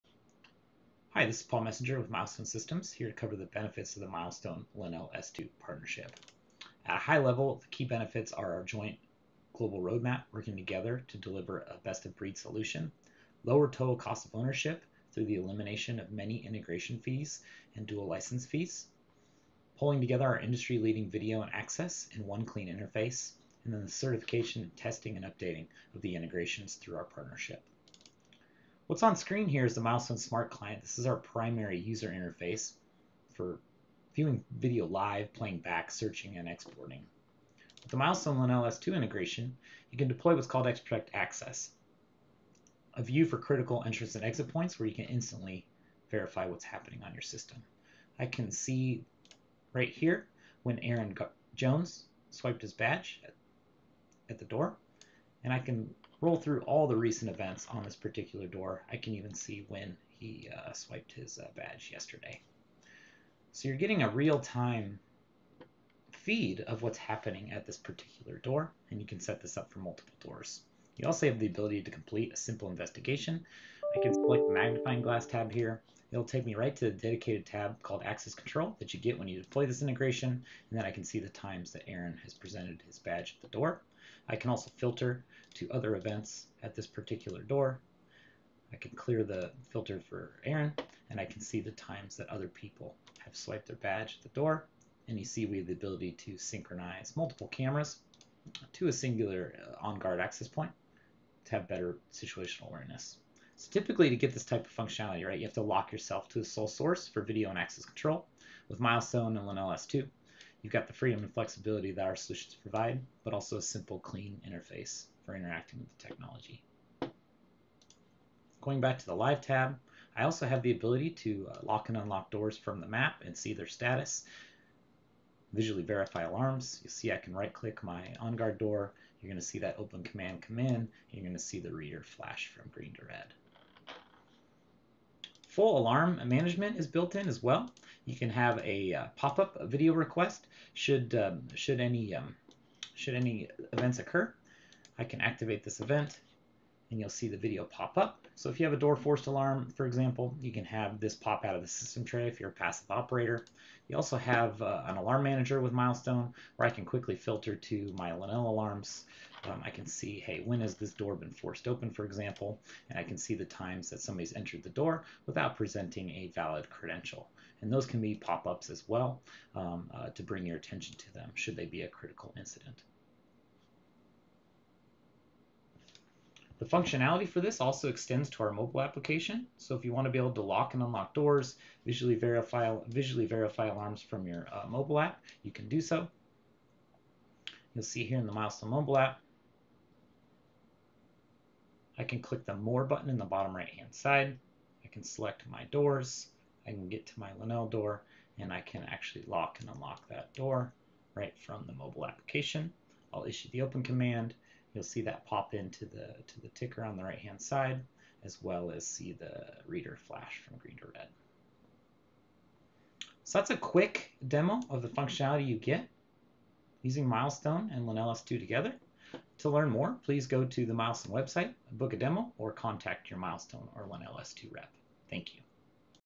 Milestone & LenelS2
This video demonstrates both Milestone and LenelS2 Access Control